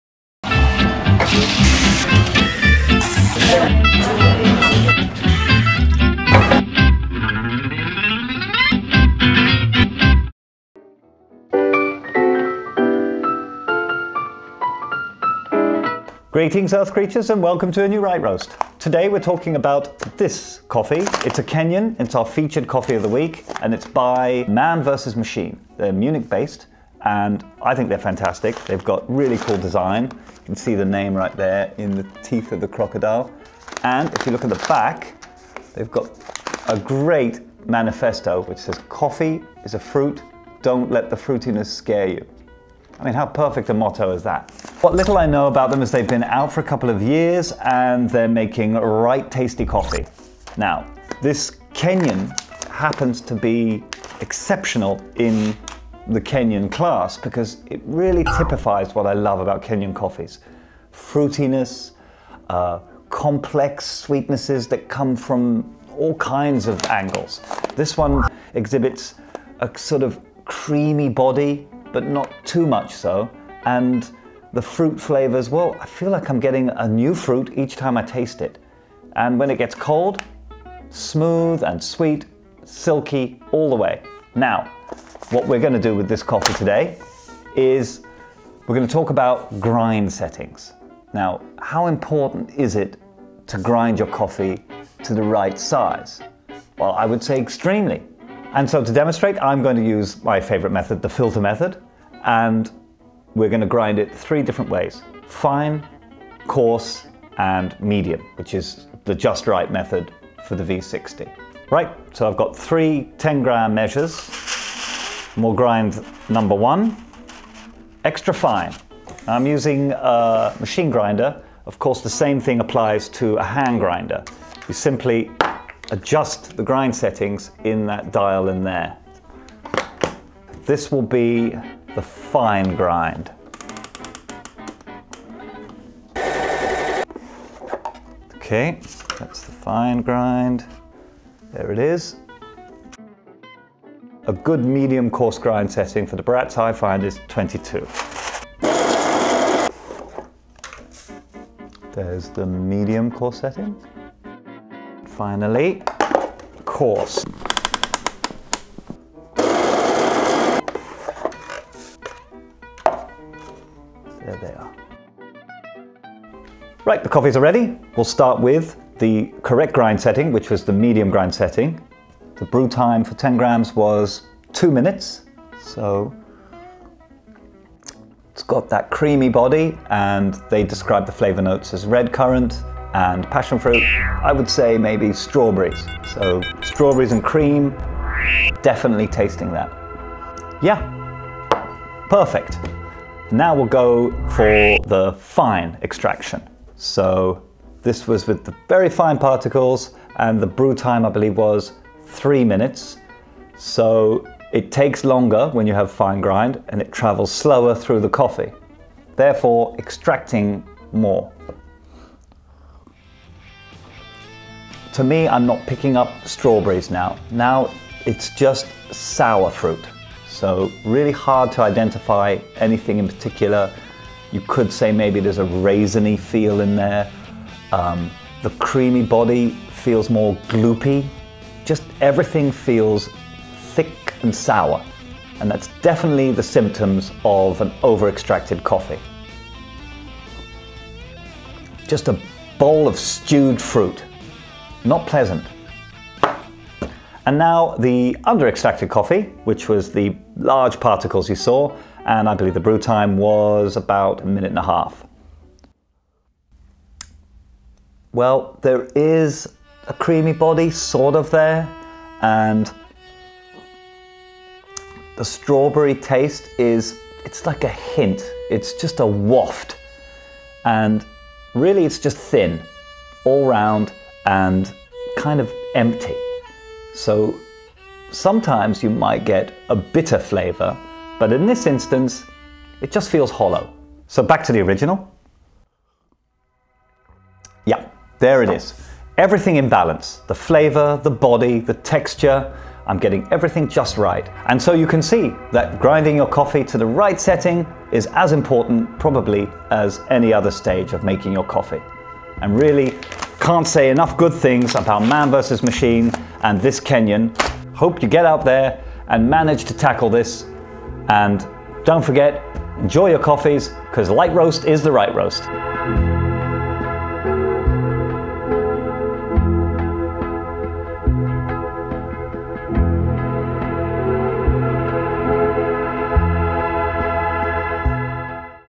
The Right Roast: Episode 59 - Know your grind feat. Man v Machine Coffee
In this episode I discuss the importance of the right grind setting. I will show how your coffee will be affected by grinding too fine or too coarse and finally, for a V60 filter, the Goldilocks of grinding just right is the medium grind.
This week's featured coffee being put to the test is a Kenyan by Munich's Man Versus Machine Coffee Roasters. It stood out for us as terrifically silky, sweet and fruity coffee with a creamy texture-making it one of the more memorable coffees of the season.

The coffee:

KIRIAINI TERESA AA
Origin: Kenya
Region: Muranga
Process: Washed
Varietal: SL28